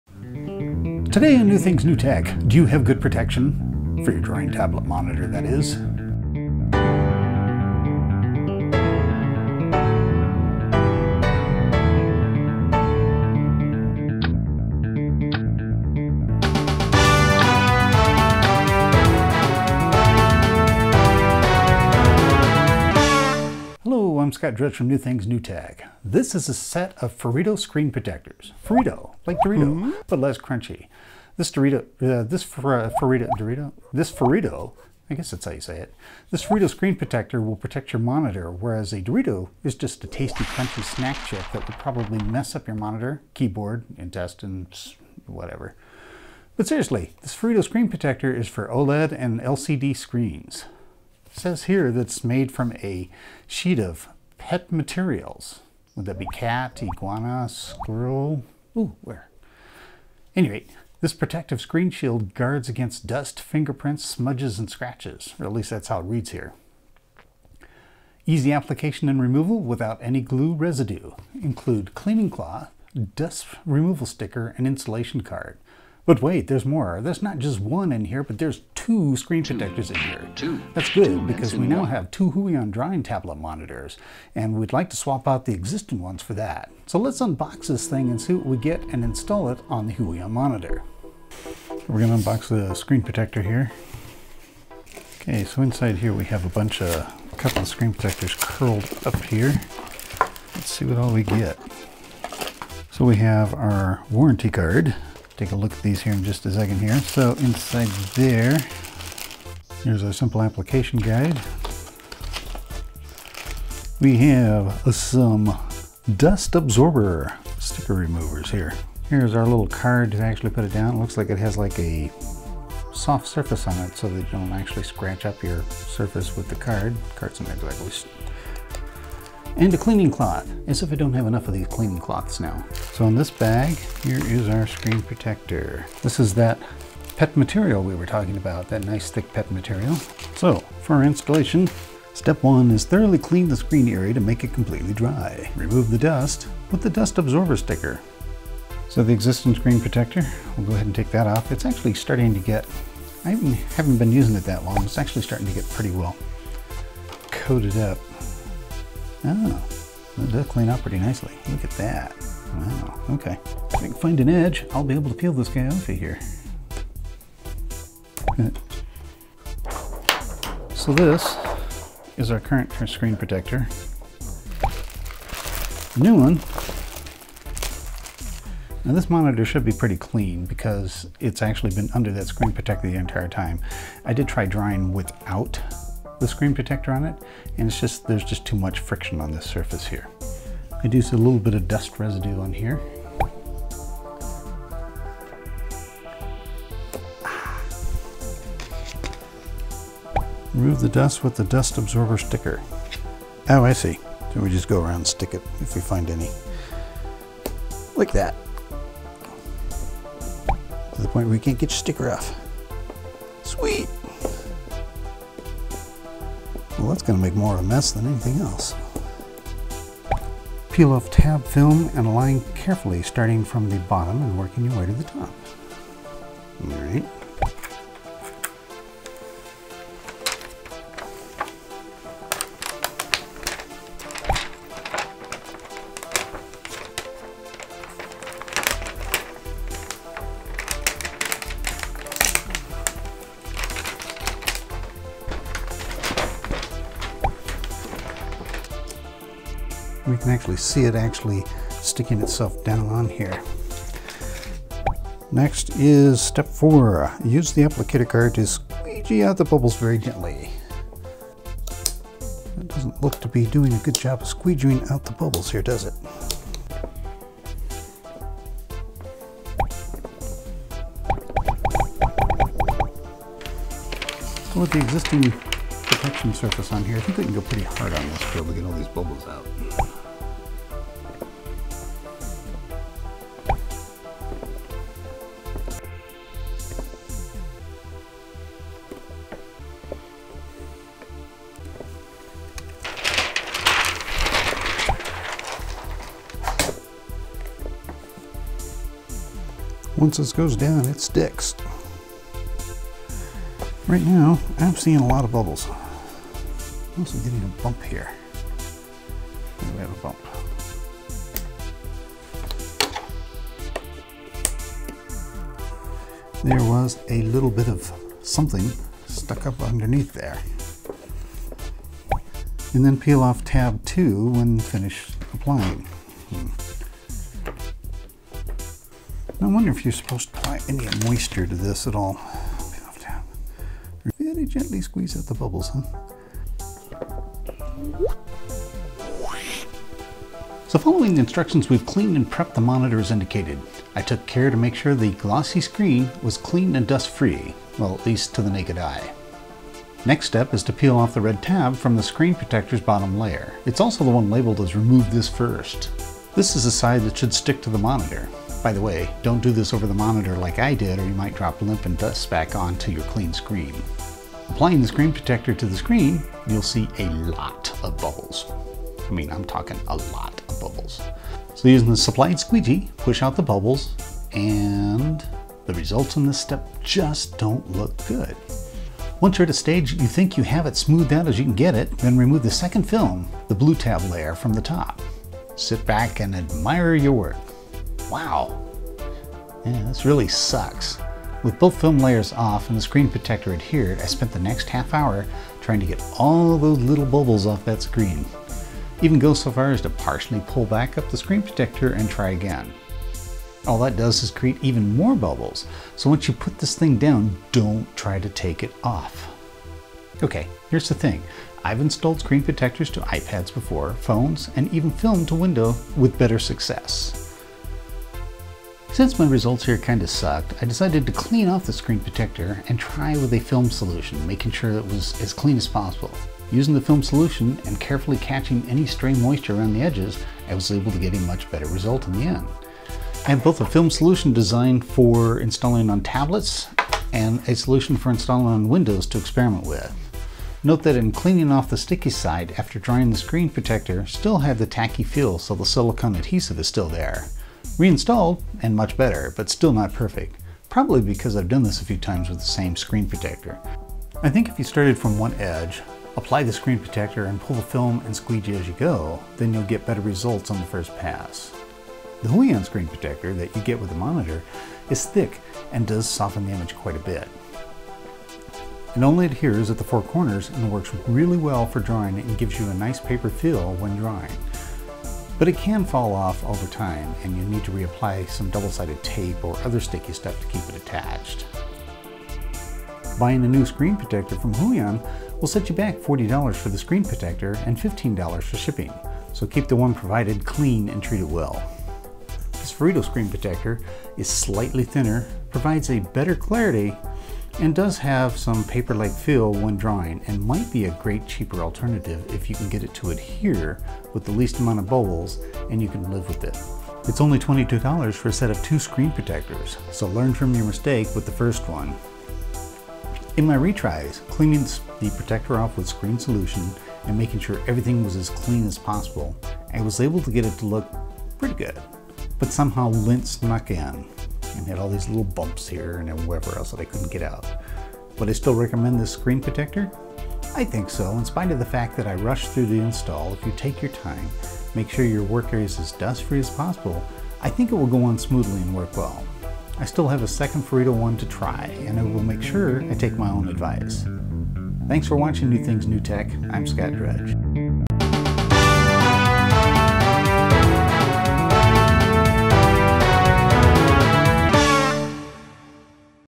Unbox & install, Review Forito screen protector on Huion monitor! Fail? | New Things New Tech
You'd think installing a screen protector is easy. Nope! Check out the issues I had installing the new Forito Screen protector on my Huion GT-220 monitor. the goal was to see how well a cheaper alternative, more clear and slightly thinner screen protector will work on a Huion drawing tablet.  Fail?

00:00 Intro
01:31 Unboxing the Forito Screen Protector
02:30 Installing the Forito Screen Protector to the Huion Monitor
05:38 Recap and Review